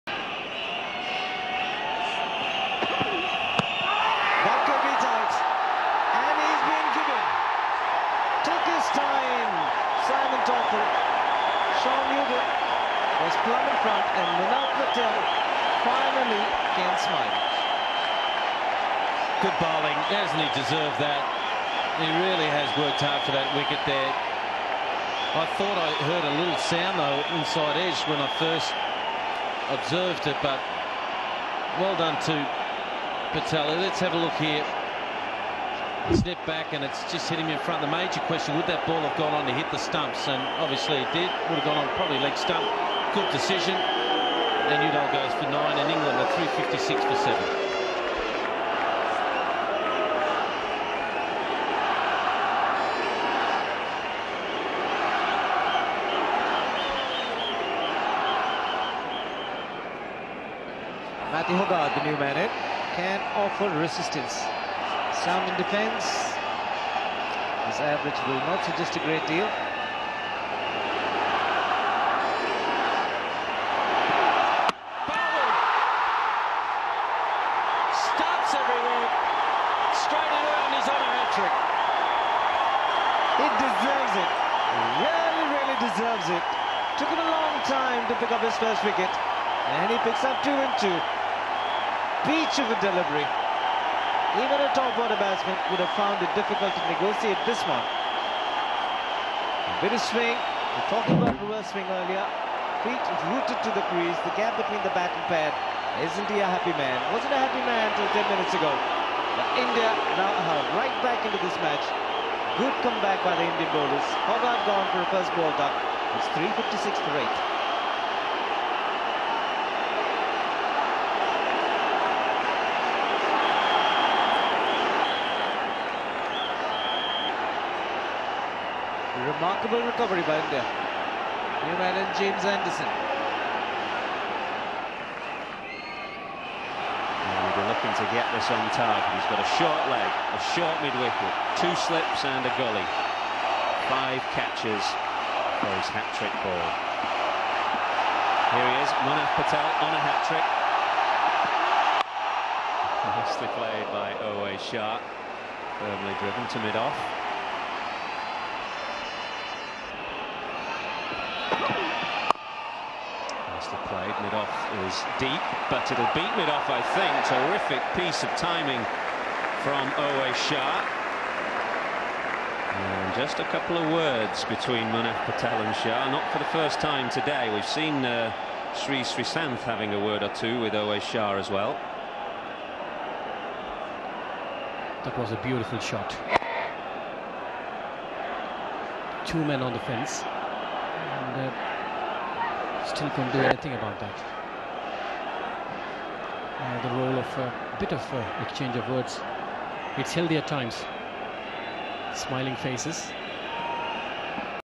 Debut match of munaf patel 🔥🔥🔥🔥
In this video munaf patel break England middle order